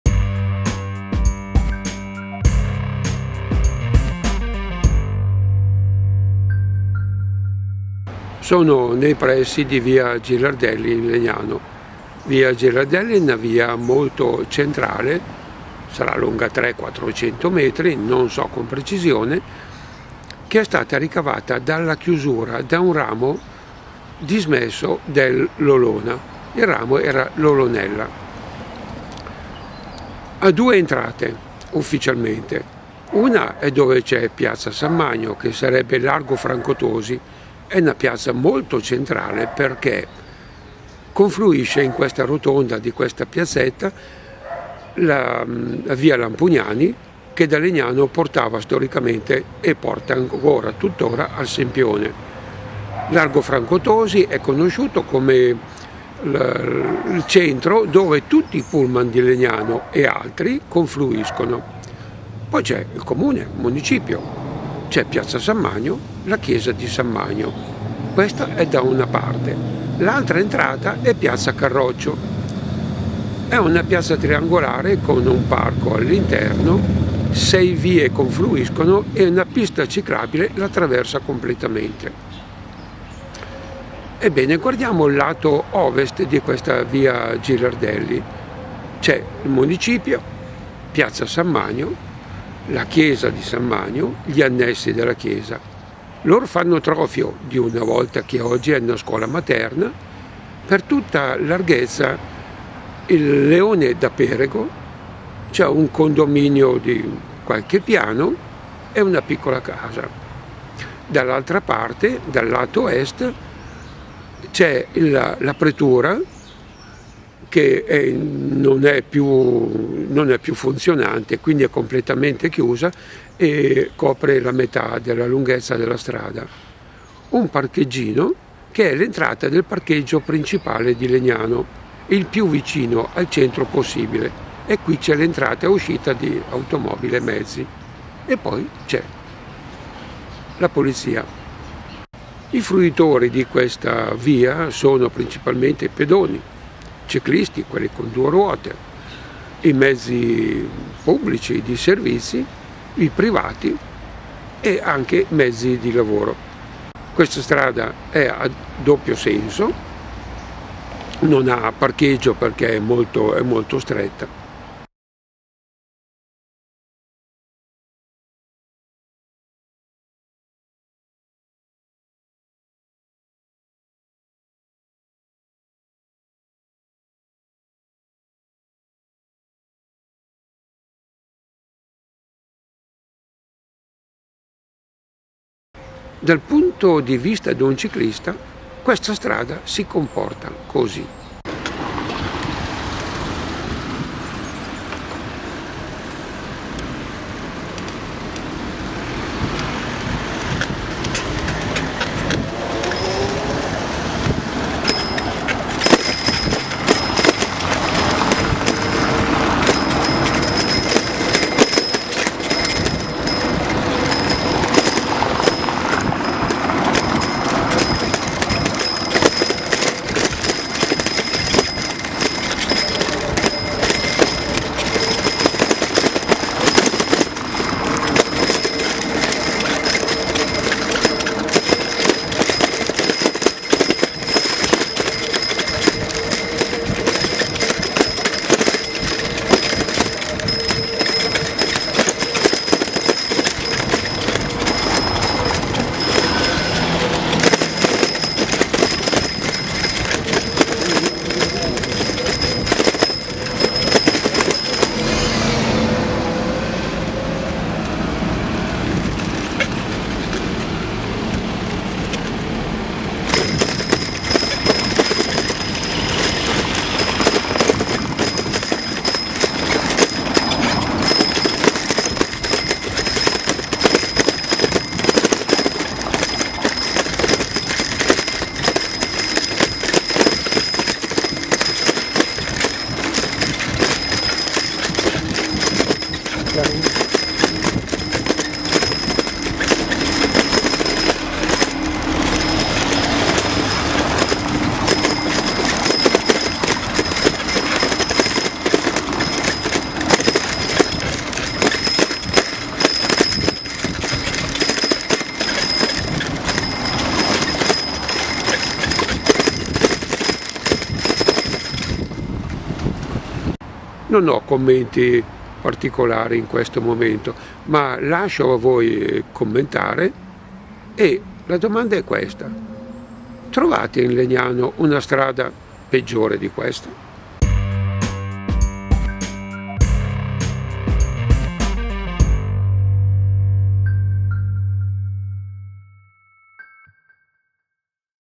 20140926 girardelli
Via Girardelli in Legnano dal punto di vista di un ciclista. Una cartolina spedita a me stesso.